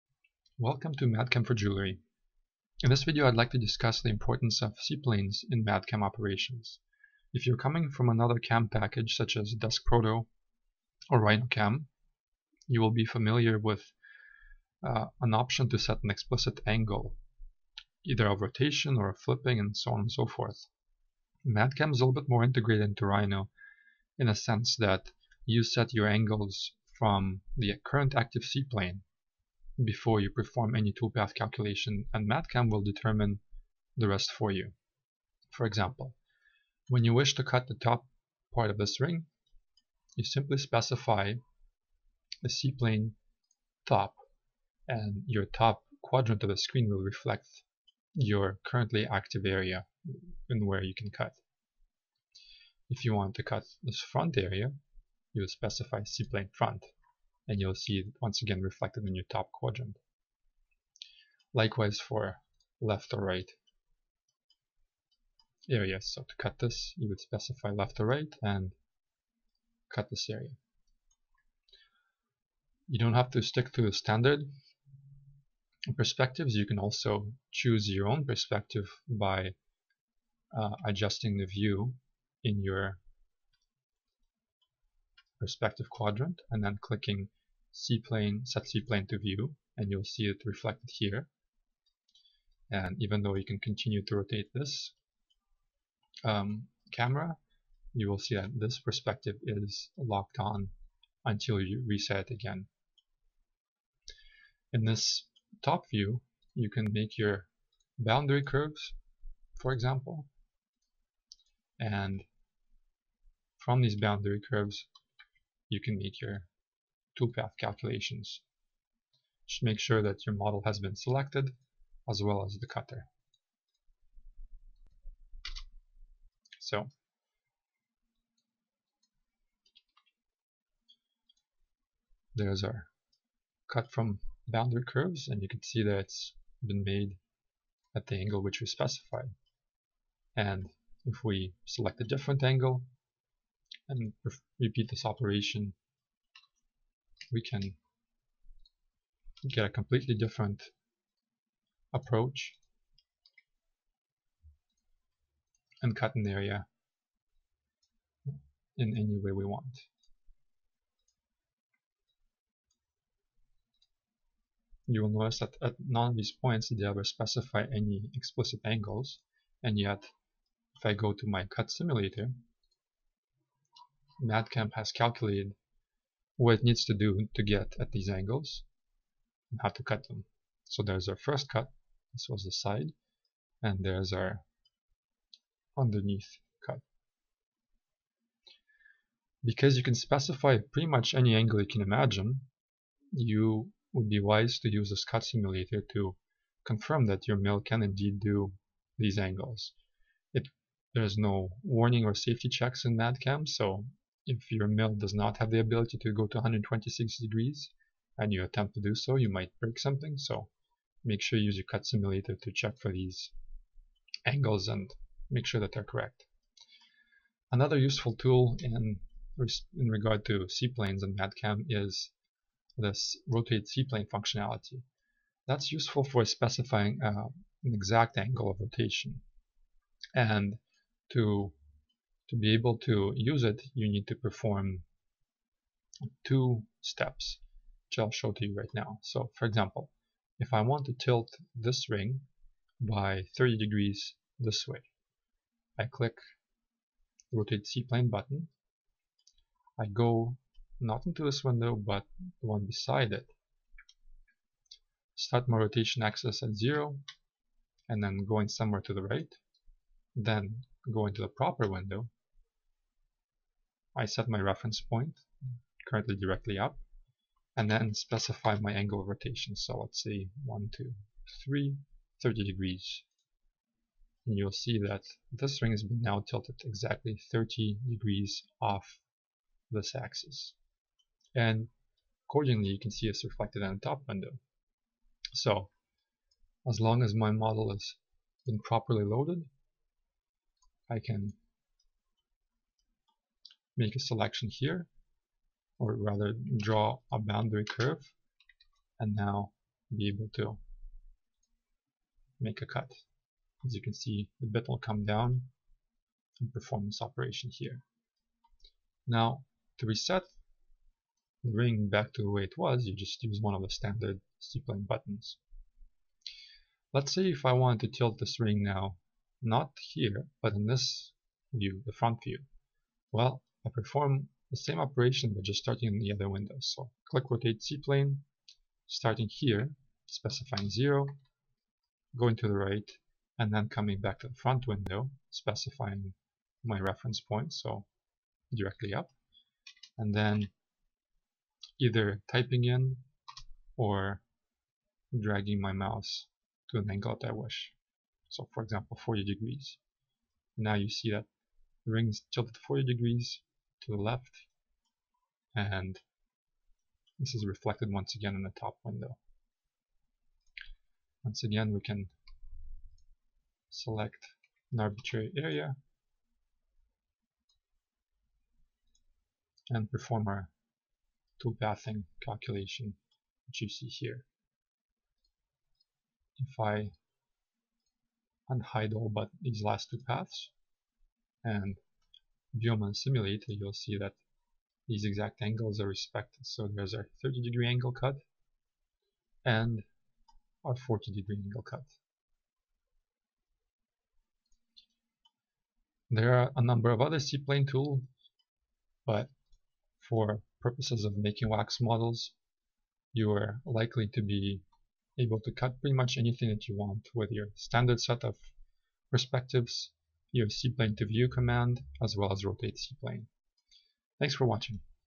madCAM for Jewelry - Lesson 9: CPlanes
How to use Construction Planes (CPlanes) to mill from any desired angle